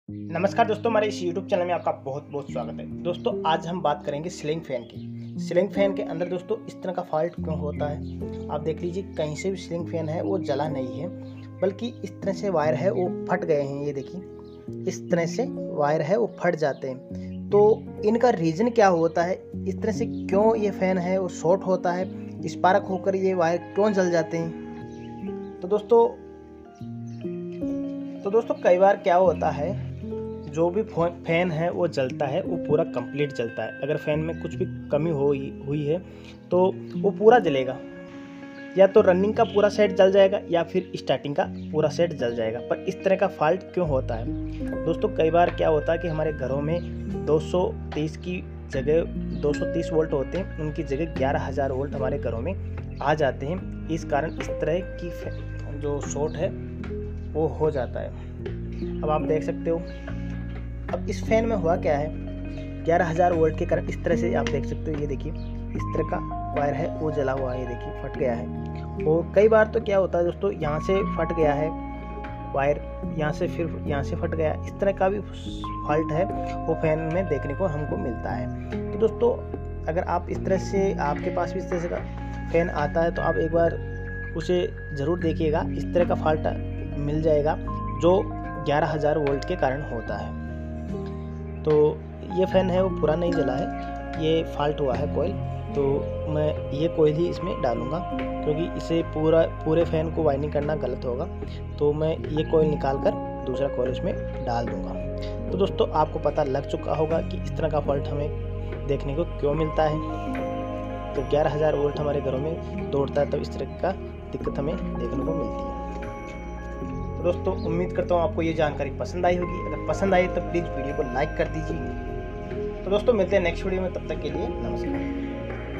सीलिंग फैन वाइंडिंग में ऐसा क्यों होता है | ceiling fan coil fault | ceiling fan winding problem
इस वीडियो में सीलिंग फैन की वाइंडिंग में कोयल होता है एक कोईल फॉल्ट होने का क्या कारण होता है वही इस वीडियो में बताया गया है उम्मीद करता हूं आपको यह वीडियो पसंद आया होगा पसंद आए तो लाइक शेयर जरूर कीजिए धन्यवाद